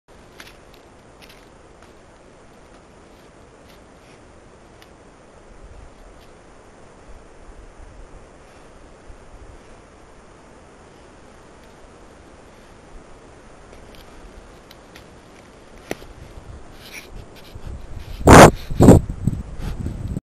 Alces alces; Elk; põder
Camera Trap Video of Alces alces; Elk; põder. Takes a close look at the camera. Sniffs at the microphone.

This species location in Taxonomy:
Metazoa / Bilateria / Chordata / Vertebrata / Mammalia / Artiodactyla / Ruminantia / Cervidae / Alces /